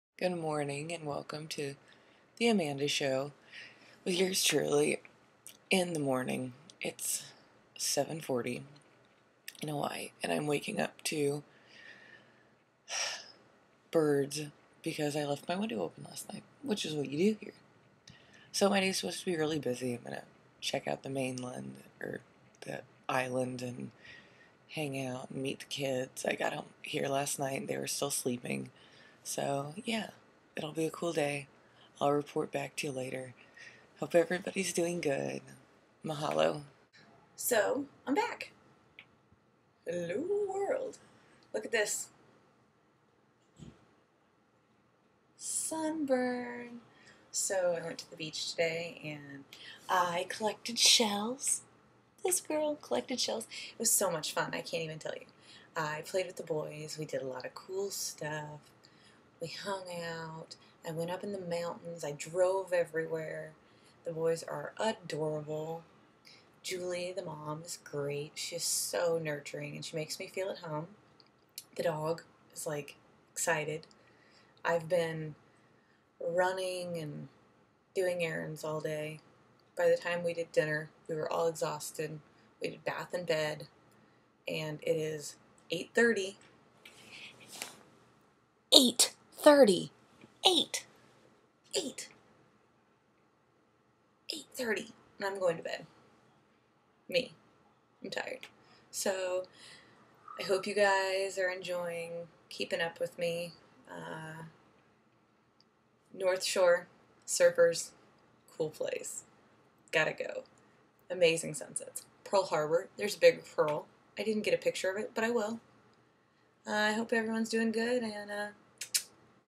My Amanda Show
First day in my new home. So cool so fast so tropical. love it.